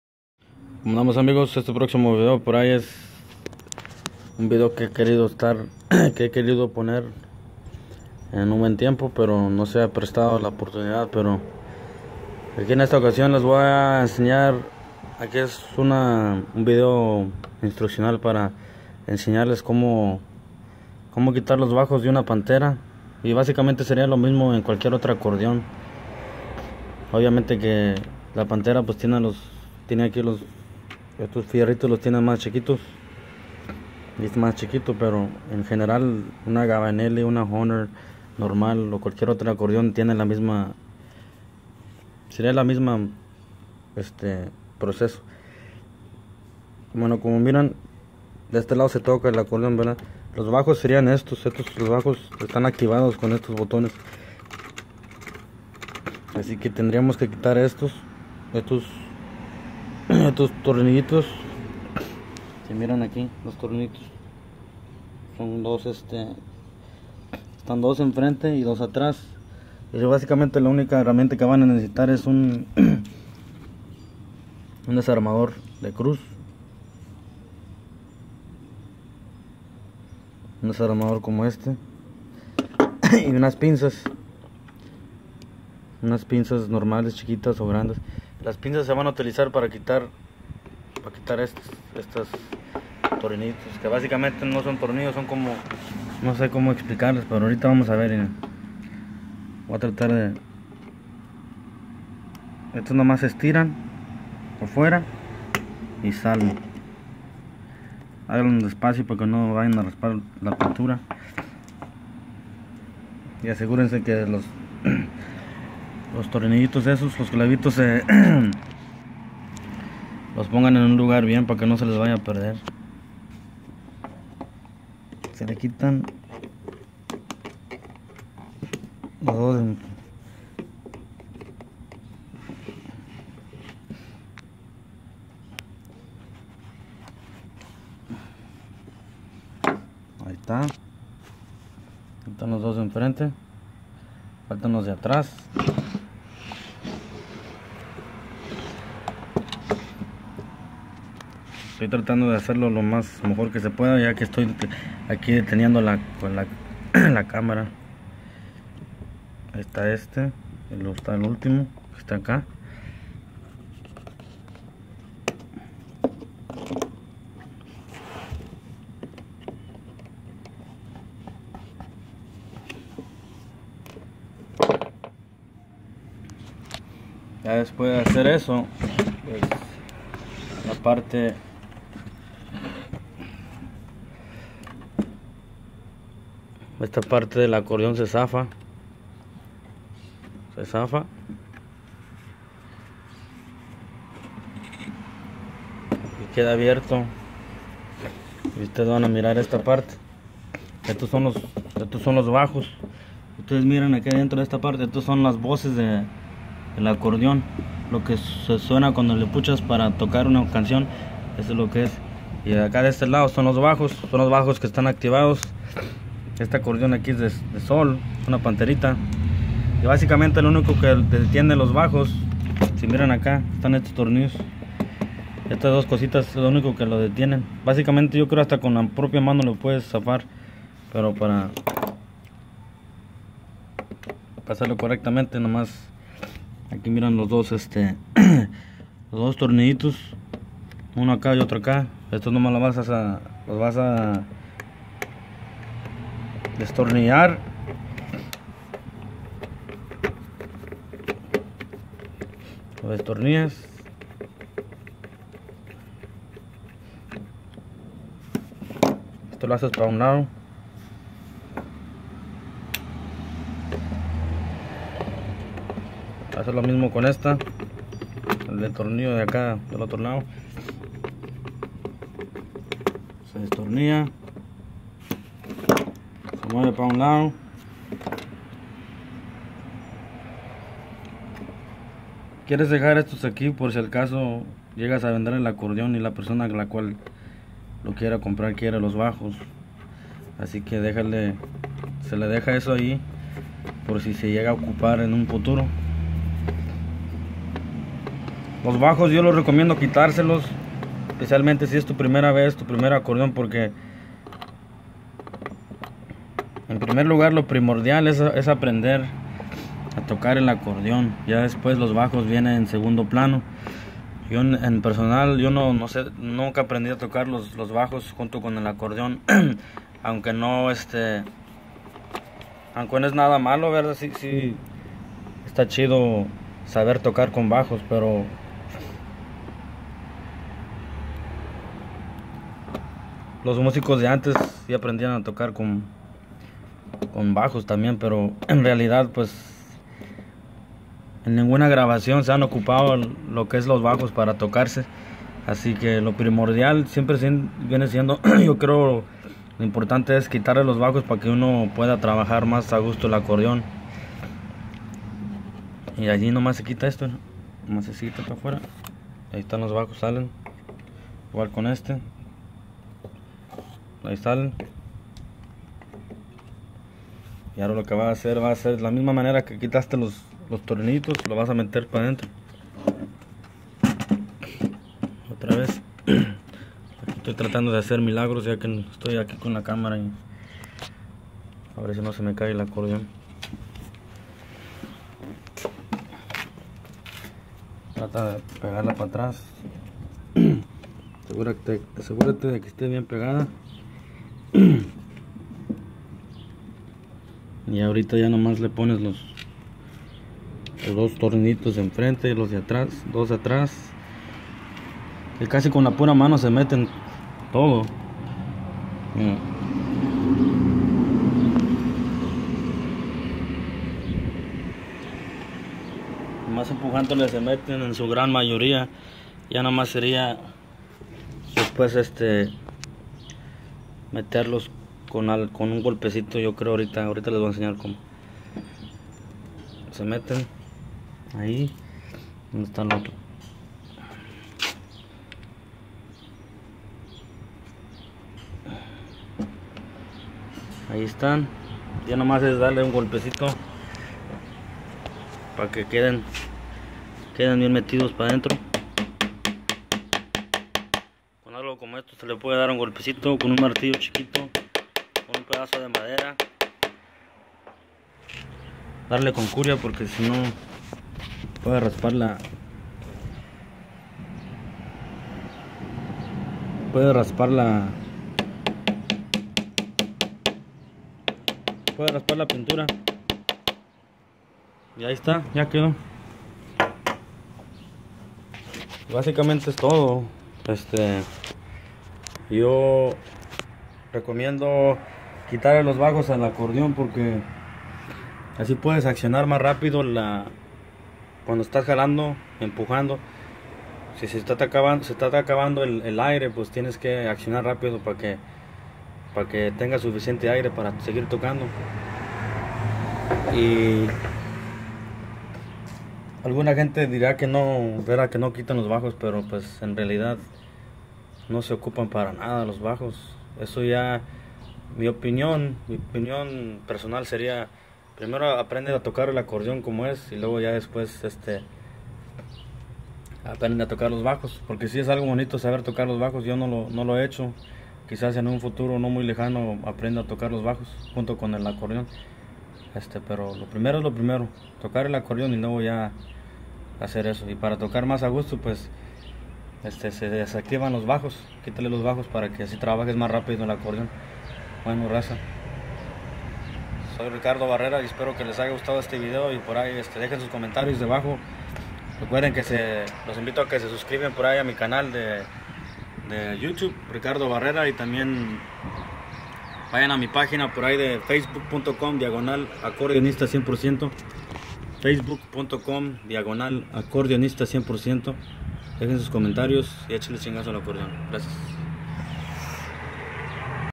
Como Quitar Bajos A Un Hohner Panther-Veanlo Muy Informativo
En este video les enseno como quitar los bajos a un Panther Hohner el cual seria el mismo proceso en cualquier otro acordeon de menos o mas calidad... Espero que les ayude en algo... Dejen sus comentarios y recuerden de subscribirse al canal para ver mas videos como este.